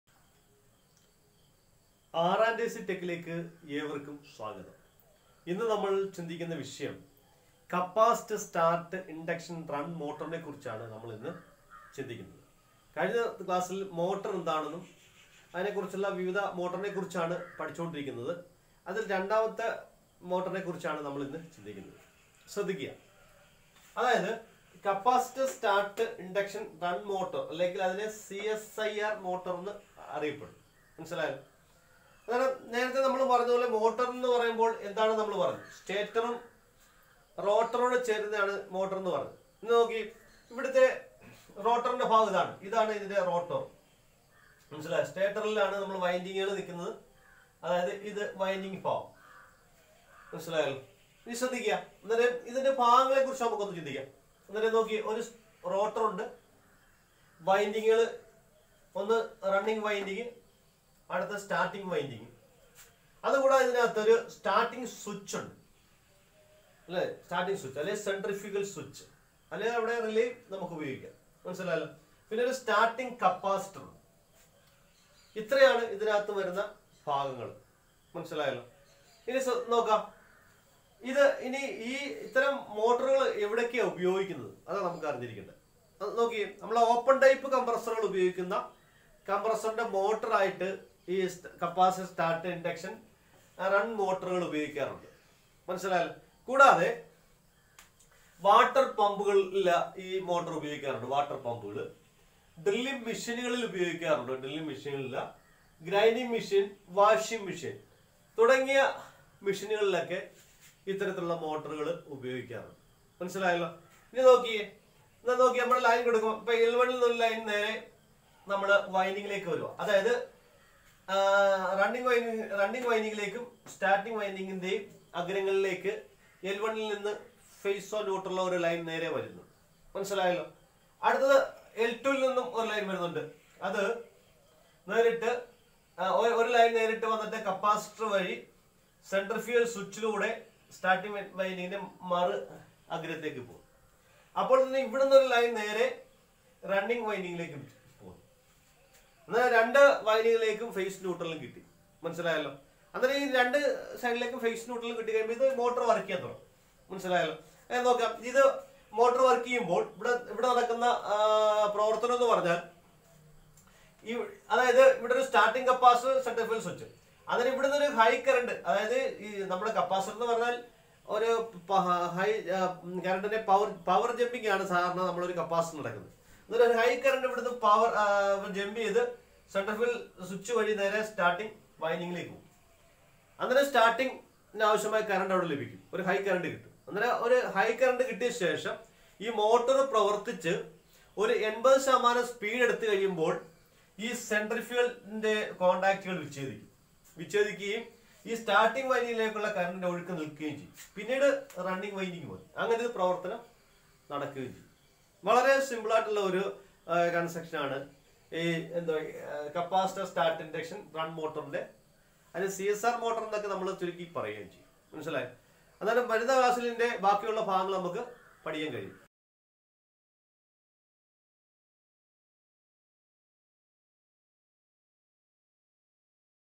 Capacitor start induction run motor (CSIR) malayalam Part 28
This type of motors are used in water cooler, deep freezer and bottle cooler. CSIR Motors are also used for open type compressor, water pump, drilling machine and grinder. A starting capacitor is connected in series with the starting winding. A starting switch or relay is used to disconnect starting winding and capacitor after starting the motor